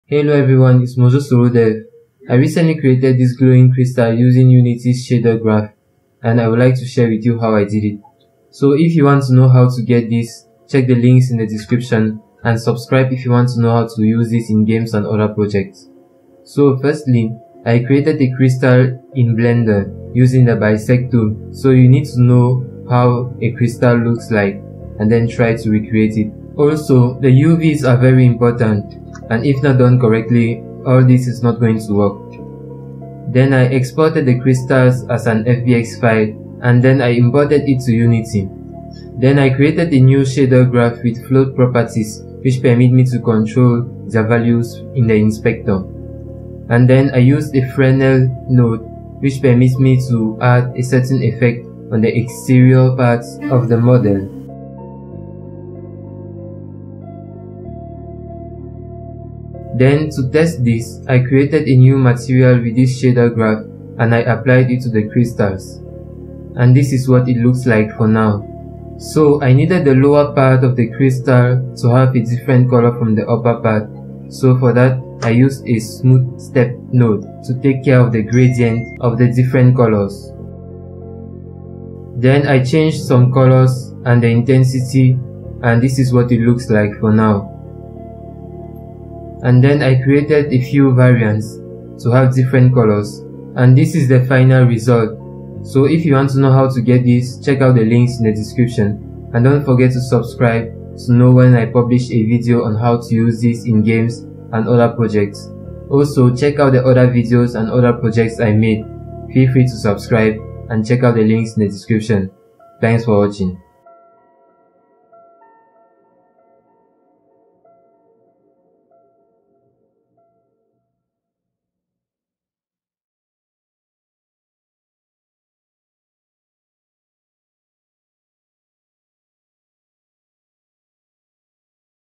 How I Created These Glowing Crystals
Prepare to be captivated by the enchanting world of glowing crystals as I reveal the step-by-step process of creating these mystical wonders using the powerful Unity Engine.